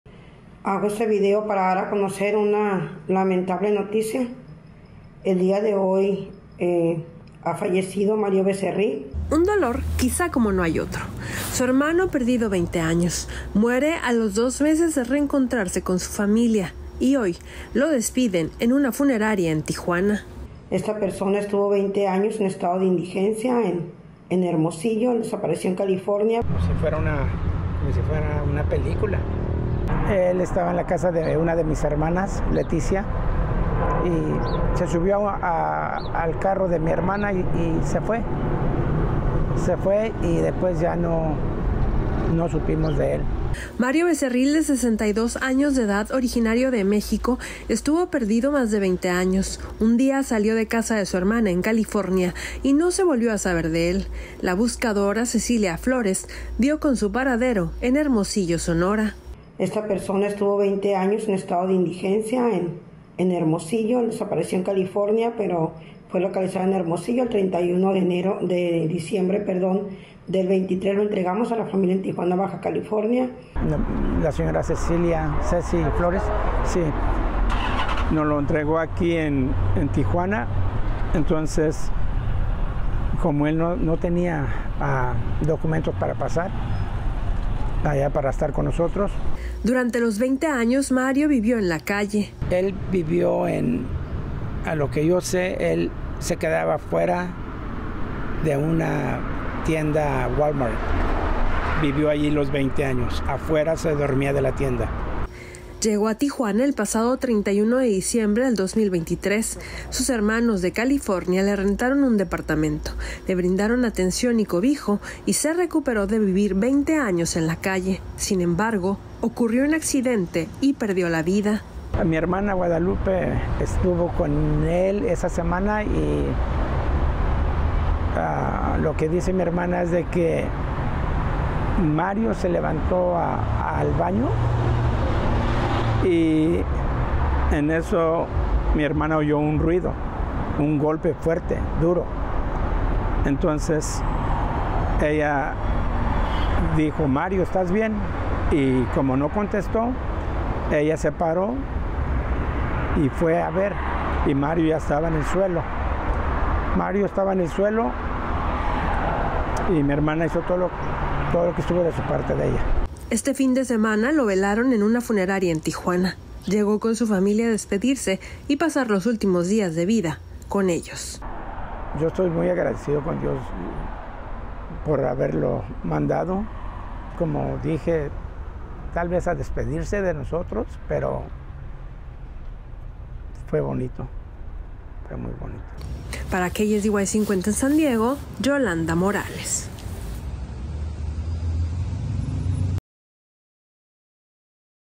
La historia de Mario Becerril
Cecilia Flores, la buscadora de personas desaparecidas, logró encontrar a Mario Becerril, quien estuvo perdido durante más de dos décadas. Sin embargo, la alegría del reencuentro se desvaneció rápidamente cuando, apenas dos meses después de abrazar a su familia, Mario partió de este mundo.

Yolanda Morales con la información.